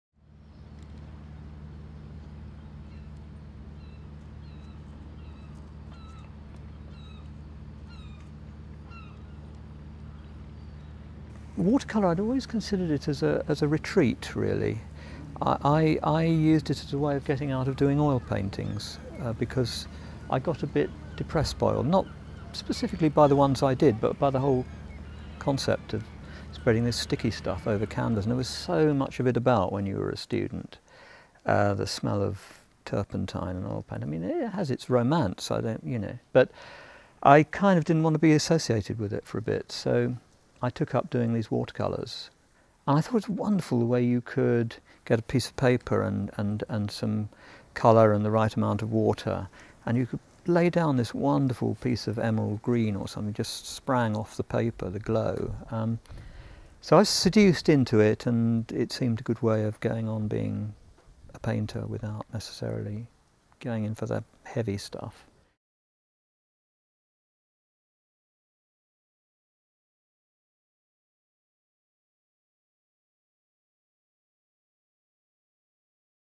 Royal Watercolour Society artist Paul Newland on painting
Royal Watercolour Society artist Paul Newland discusses his work.